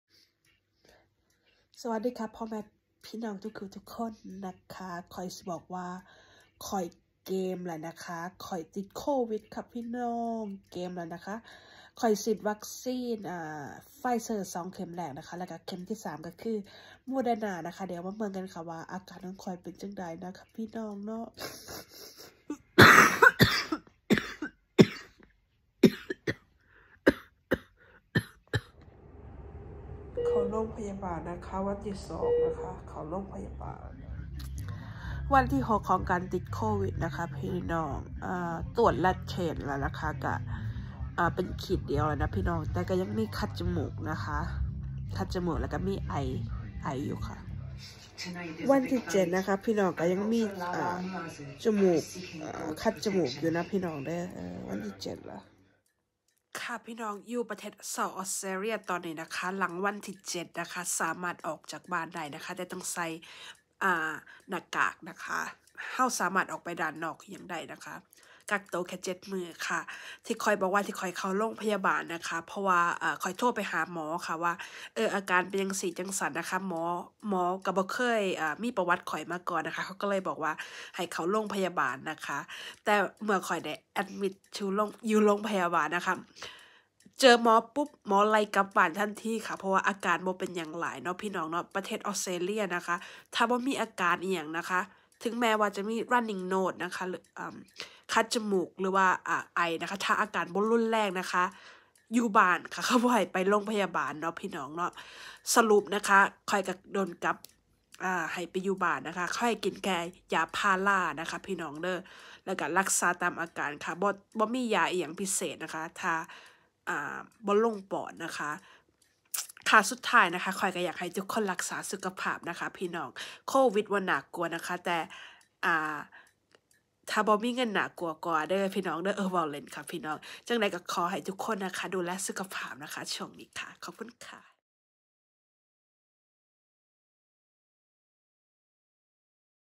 ติดโควิด ที่ออสเตรเลีย หมอไล่ให้กลับบ้าน
แชร์อาการติดโควิด ค่าโรงพยาบาลฟรี….ค่ายาพาราไป.ซื้อเอง …vdo นี้เป็นแค่การแชร์ประสบการณ์เท่านั้น ไม่มีเจตนาพาดพิงถึงใคร sharing is caring ….stay safe